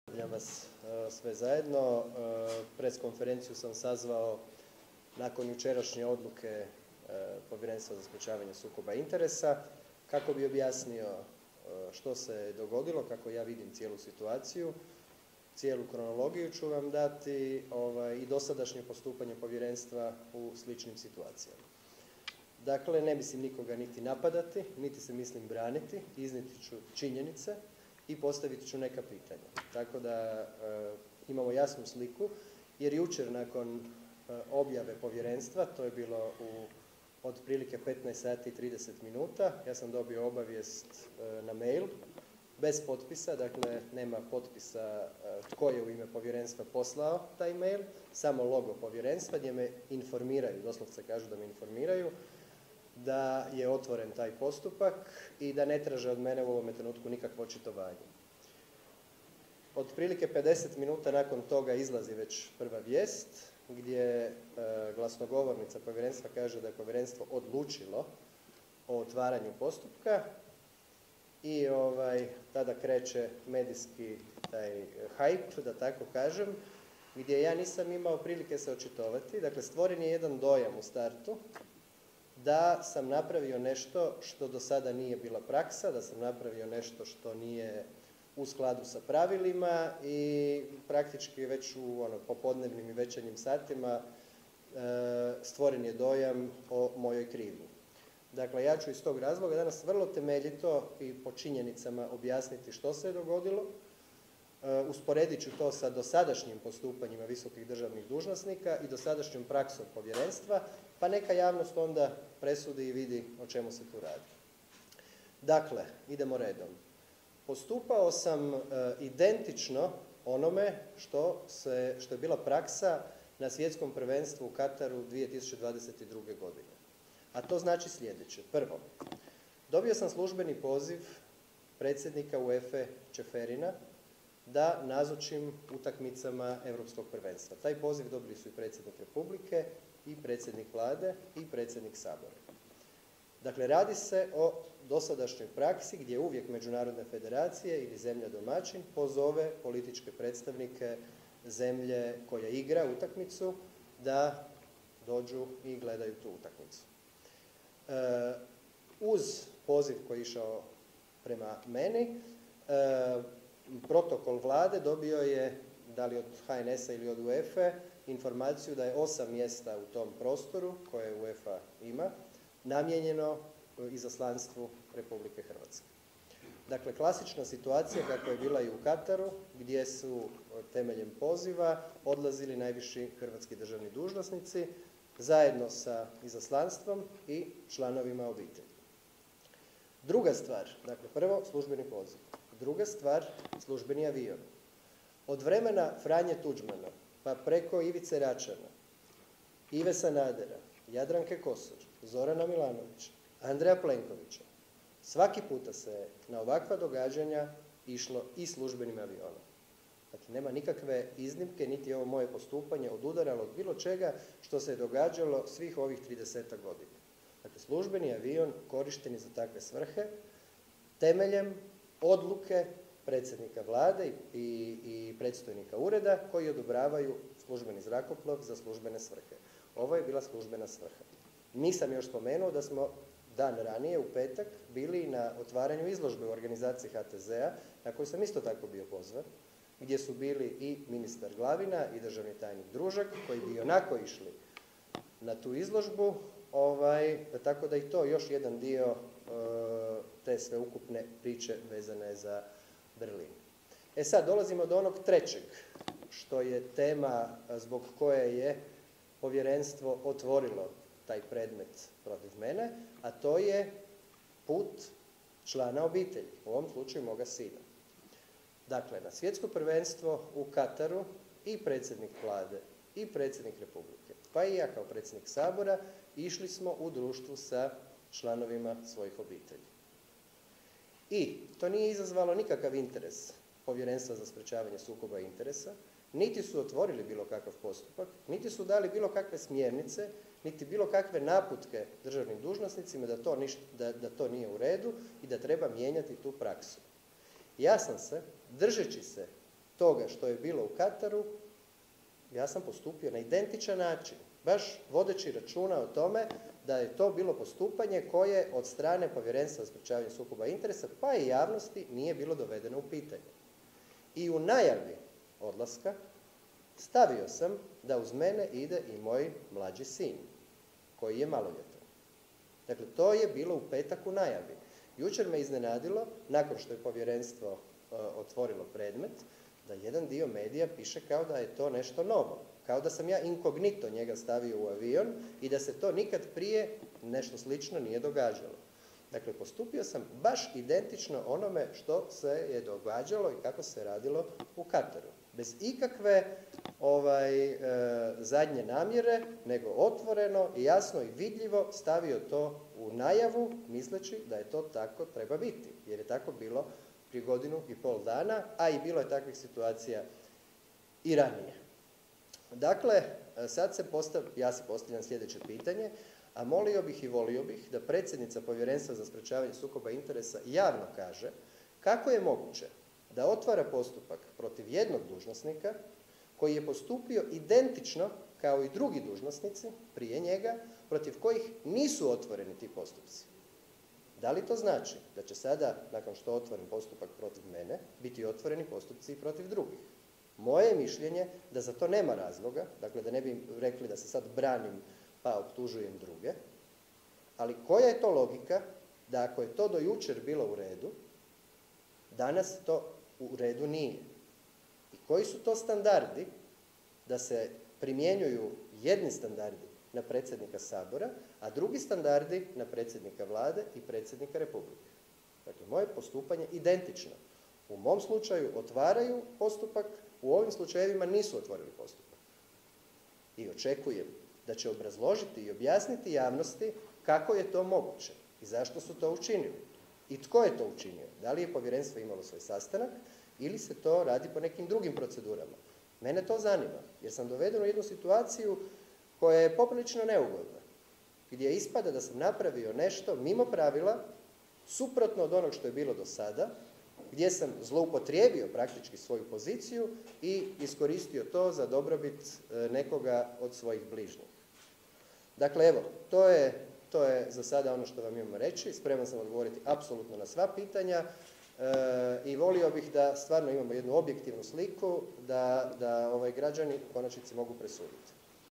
Jandroković o putu u Berlin: "Povjerenstvo je otvorilo predmet zbog puta mog sina"
Gordan Jandroković sazvao je konferenciju za medije povodom njegovog leta službenim avionom na utakmicu u Berlin.